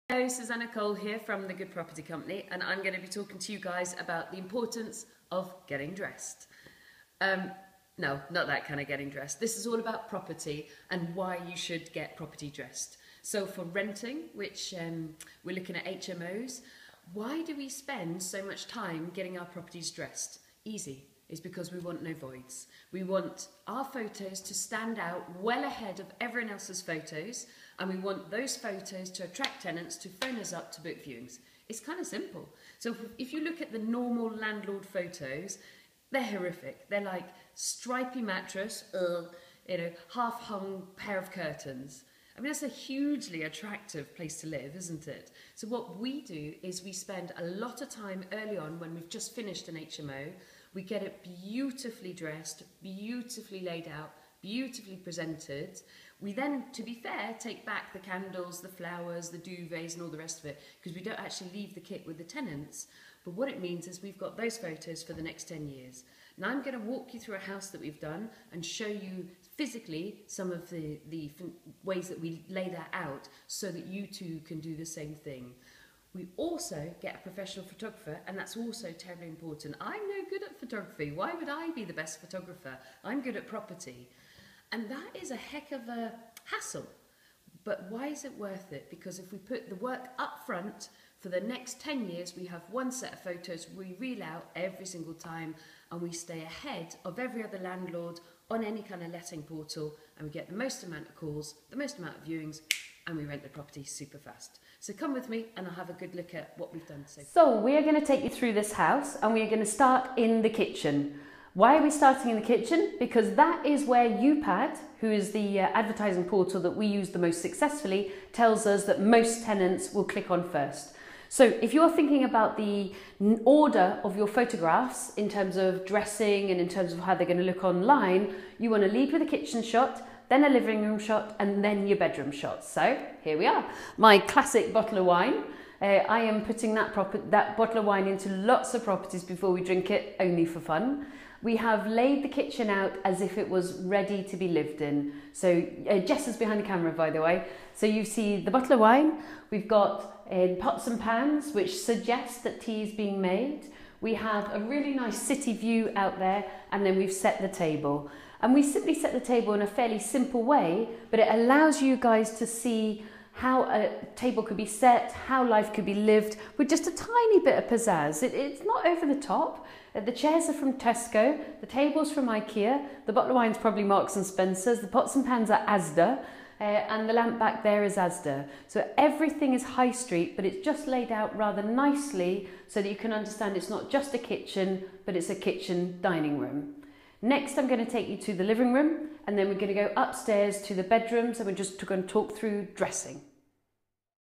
How To Rent HMO's Fast!
In this video, Susannah Cole from TGPC talks about the importance of getting dressed!

Dressing a property is essential when it comes to renting out an HMO fast. Staging a newly renovated home with furniture and other cozy objets means that you can take stunning photos that will last you up to 10 years!

And having beautiful photos makes your property stand out amongst the competition. Because remember, professional photos of a dressed home attract premium tenants and ensure your property is rented in no time!

If you want to learn how to make serious cash in property and get exclusive access to property training workshops and mentoring programs, be sure